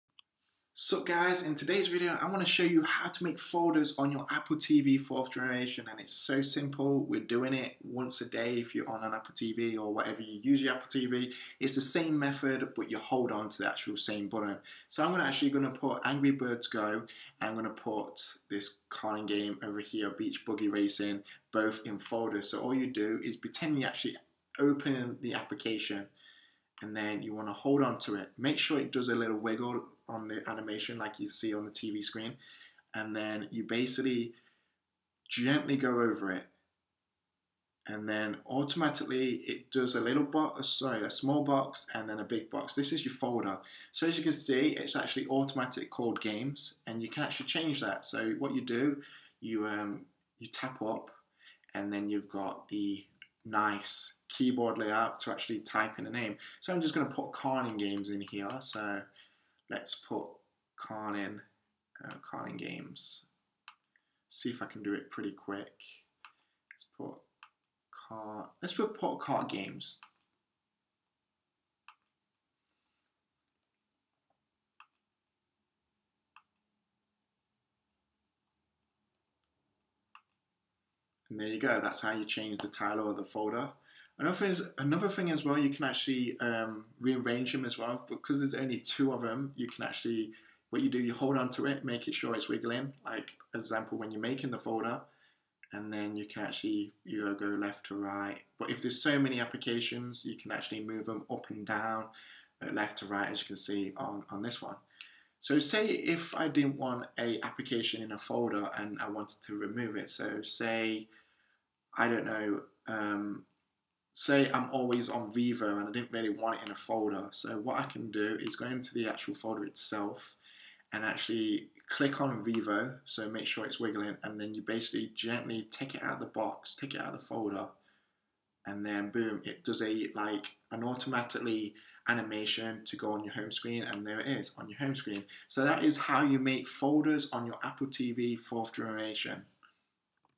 how to make folders on latest update Apple tv 4th gen ??????
in this video i will show you how to create folders on latest update Apple tv 4th gen and how to remove apps or games out of folders